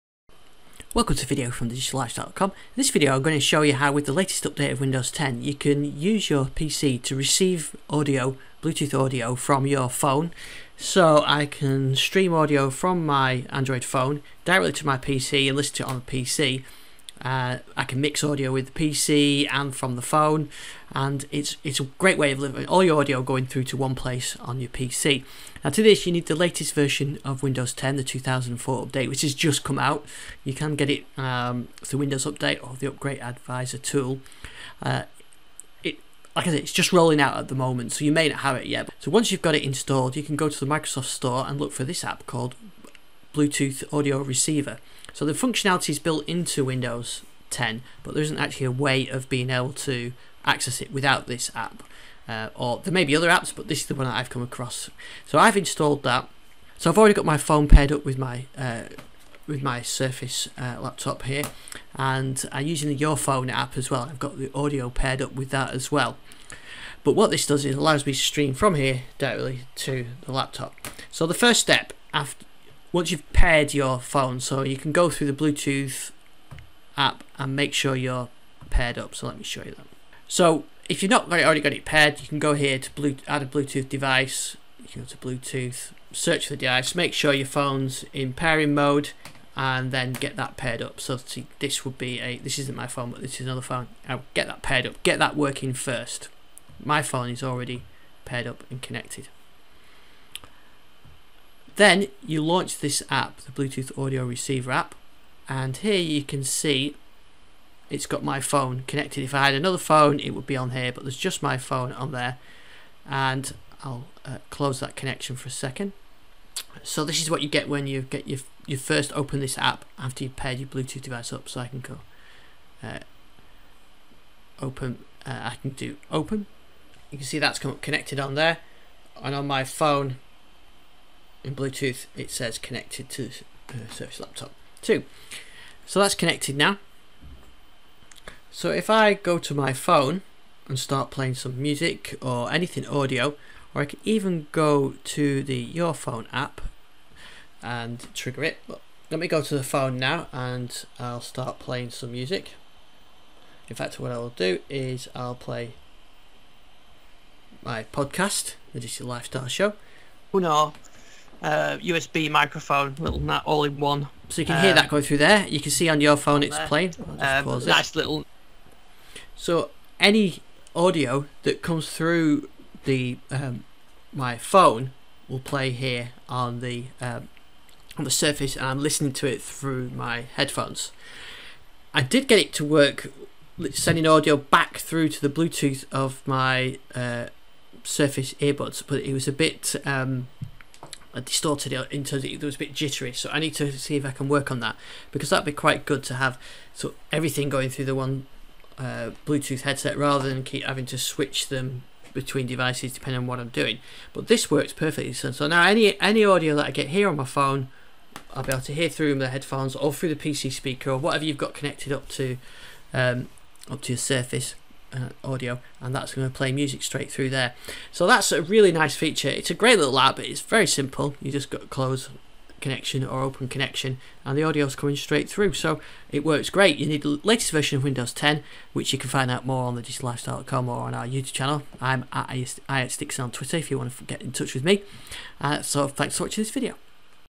How to stream music from an Android phone to Windows 10
With the Windows 10 May 2020 Update Microsoft reintroduced Bluetooth A2DP support into Windows 10. This means you can stream audio from Bluetooth device to Windows 10, so you could stream music or podcasts directly to your PC and then it can playback via your PC’s speaker or headphones.
To get started you will need the Windows 10 May 2020 (version 2004) update. Microsoft have just started rolling out the update which you can read more about on my May 2020 Update page, there a couple of ways of getting the update (see my guide).
Once you have the Windows 10 May 2020 Update you will need a receiver app, the one I found is called Bluetooth Audio Receiver and is free from the Microsoft Store.